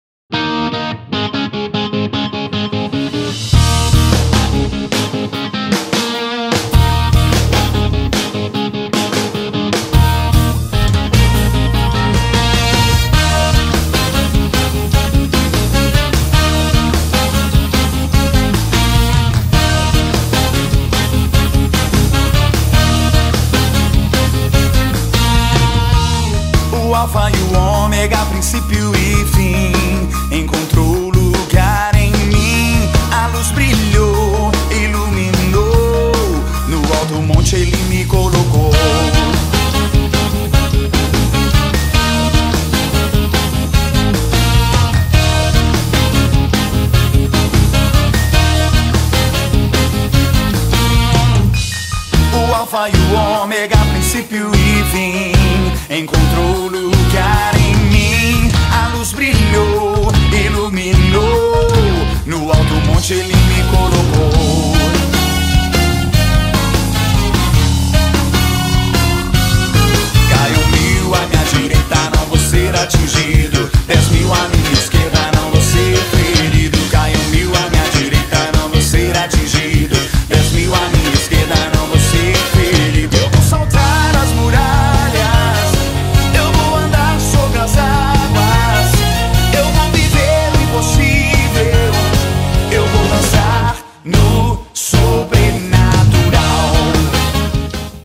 ANDRE VALADAO - JAGUARE E SAO GONÇALO
MELHORES MOMENTOS DA TURNE SOBRENATURAL DE AV NO ESPIRITO SANTO E NO RIO DE JANEIRO.